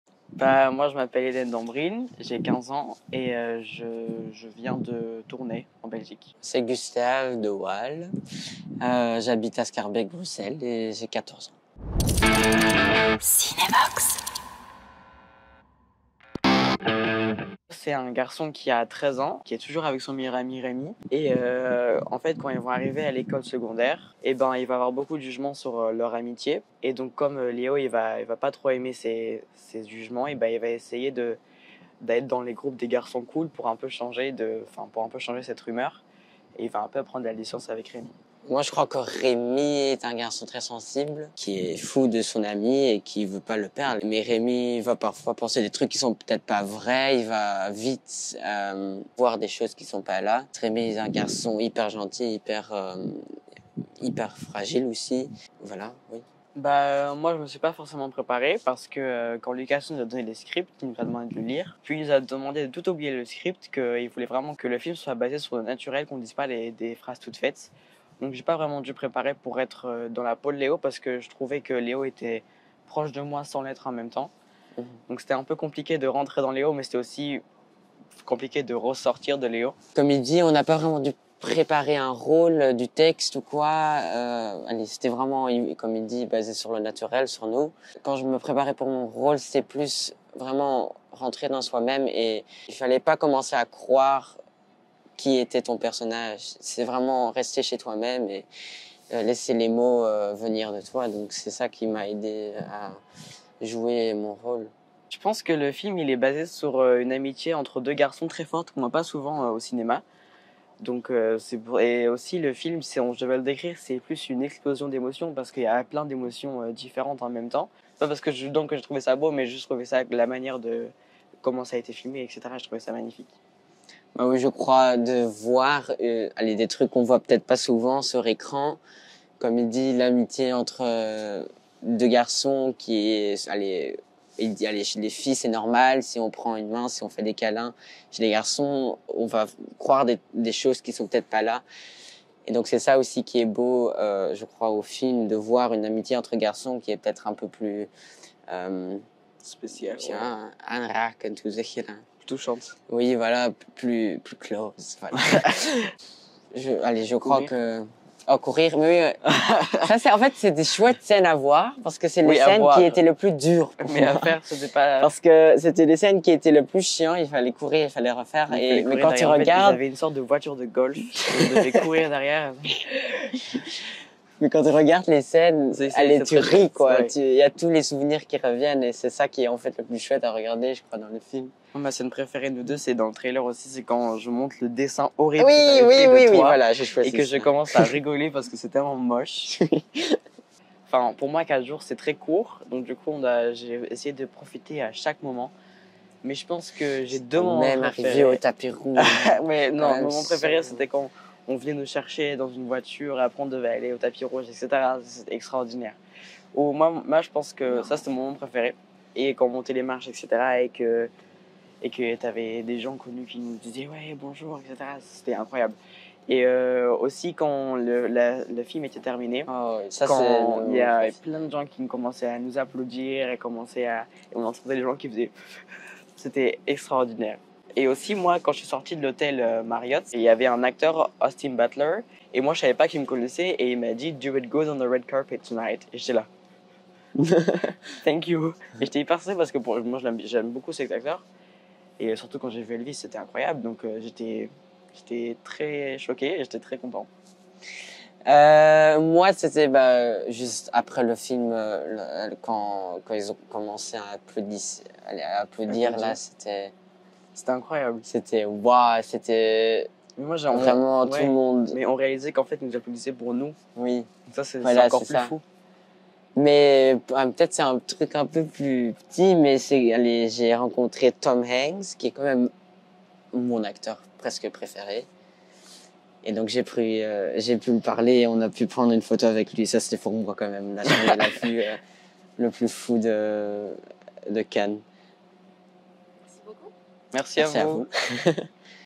Rencontre avec les jeunes héros de "Close", Eden Dambrine et Gustav De Waele
Rencontre avec Eden Dambrine et Gustav De Waele, les deux héros de "Close", le nouveau film de Lukas Dhont, Grand Prix du Jury à Cannes en mai dernier, qui sort ce mercredi 2 novembre en Belgique.